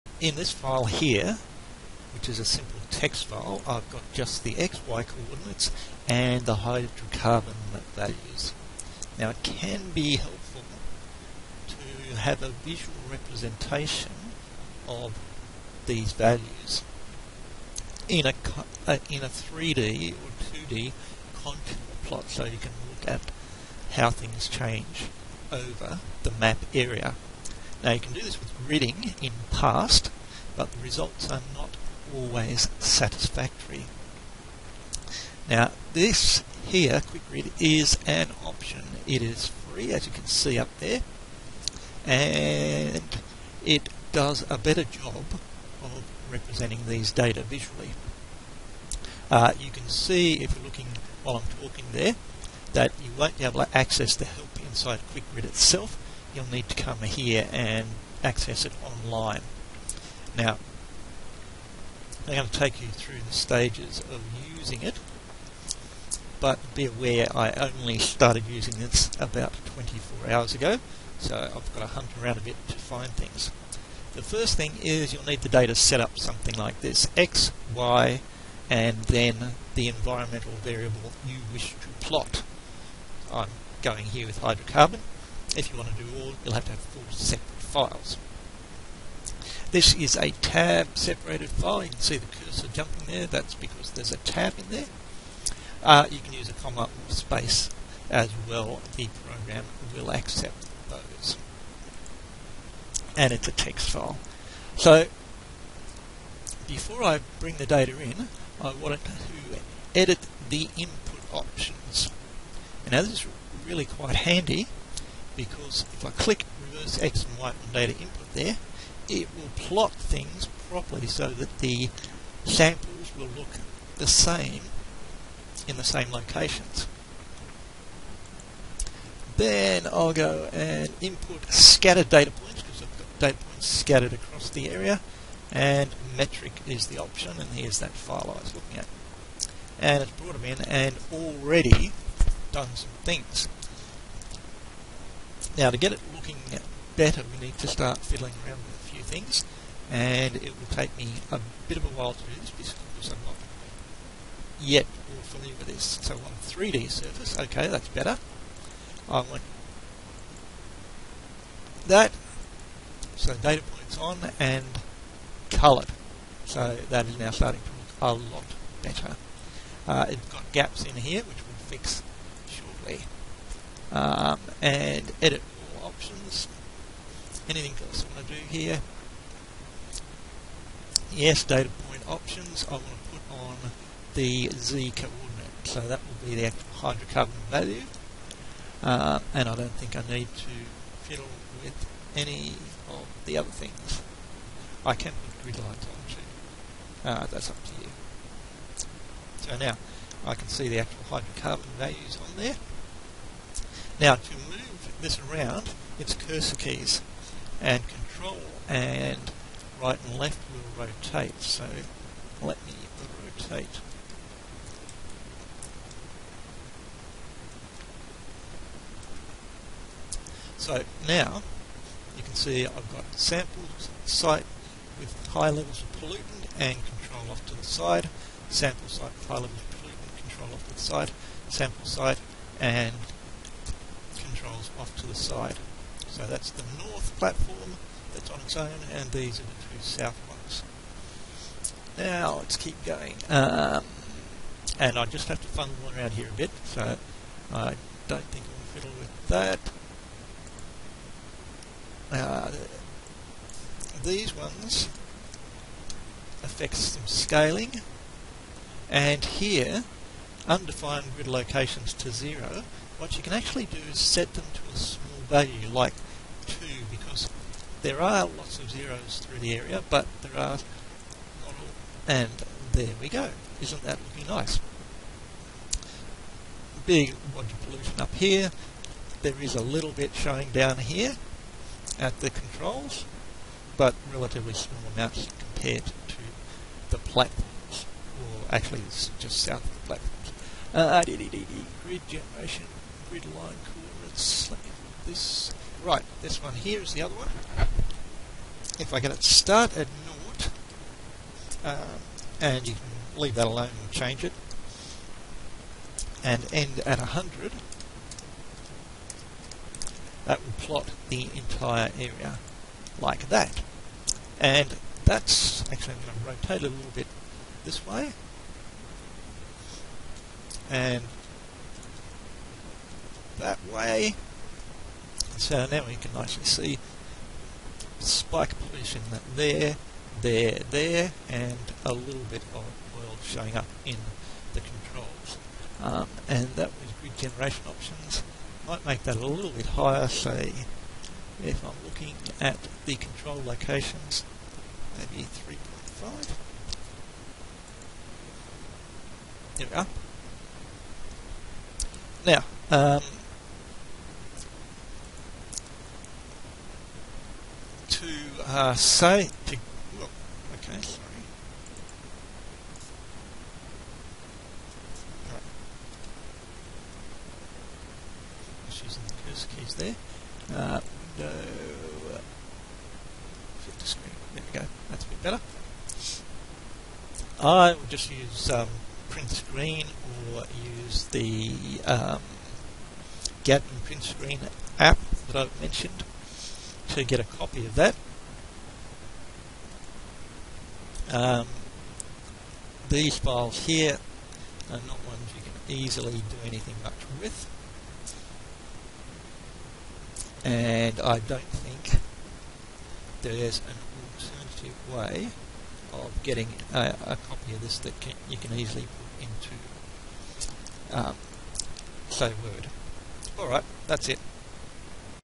3D contour plots with QuikGrid
A short video showing how to create nice 3D contour plots of the environmental variables using QuikGrid.